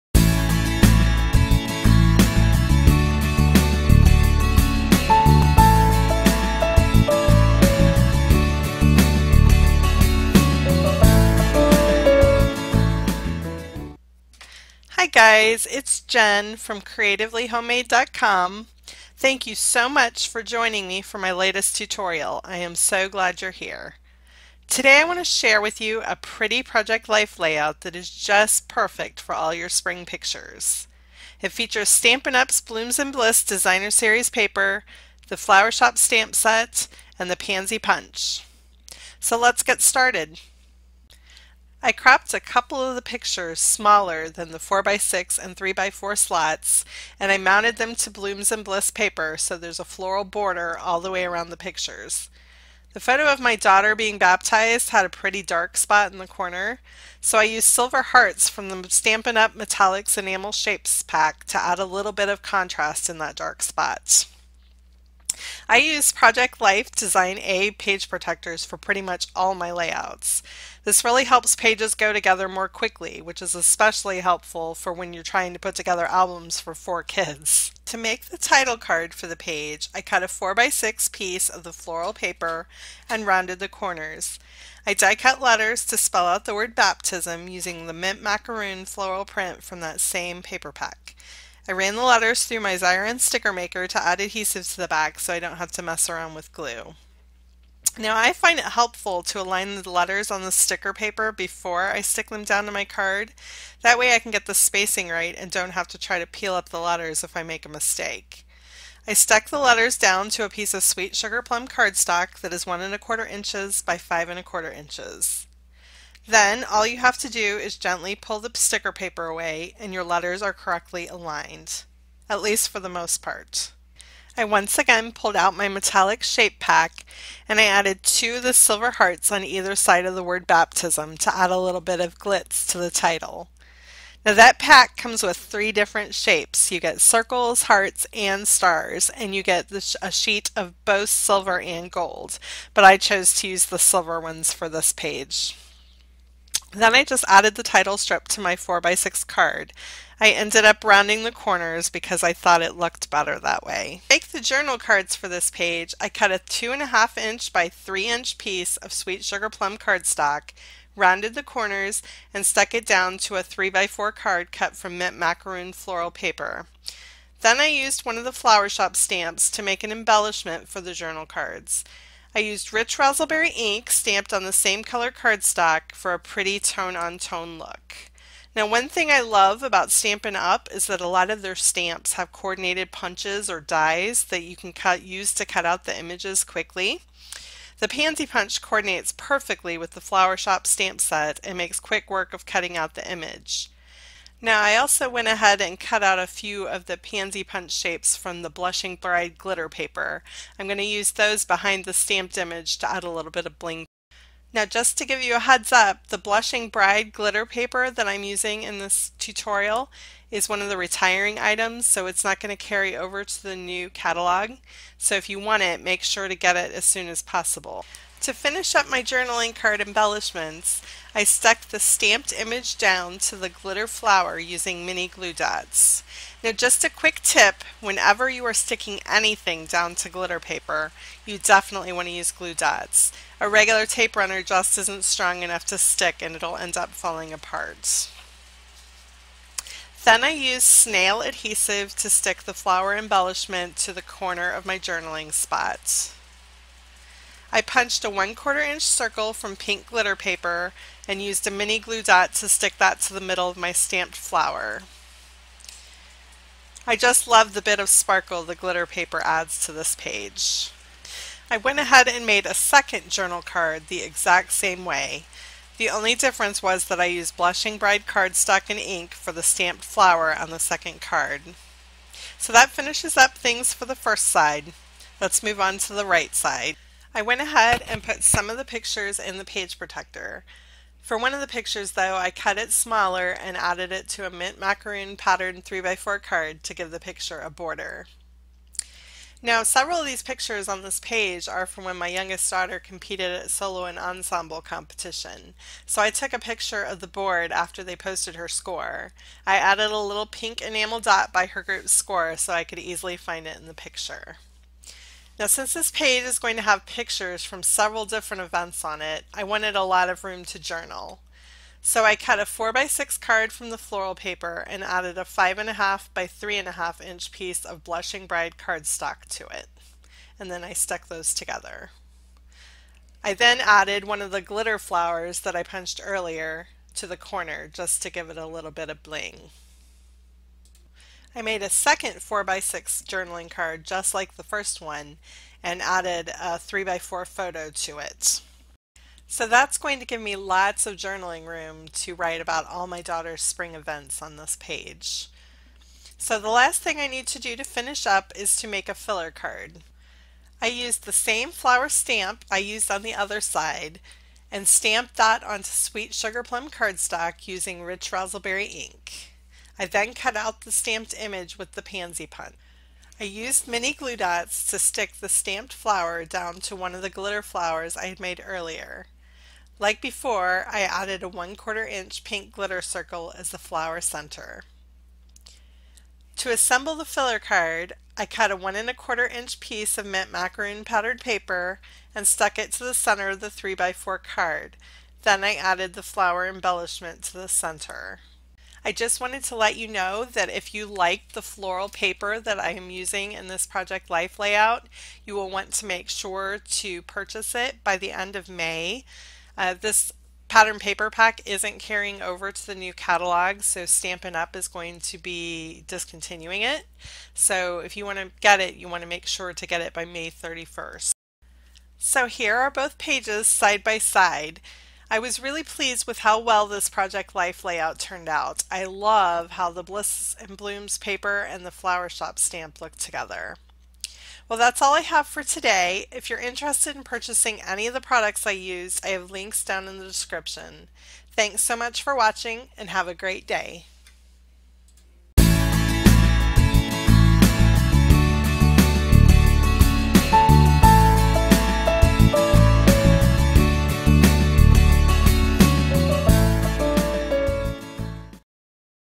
Spring Project Life Pages Featuring Stampin Up's Blooms & Bliss Paper
Tutorial for making a pretty Project Life Spring layout using the Stampin Up Blooms & Bliss Designer Series Paper and the Flower Shop Stamp set.  I show how I make Project Life journal cards from pretty patterned paper and add stamped embellishments.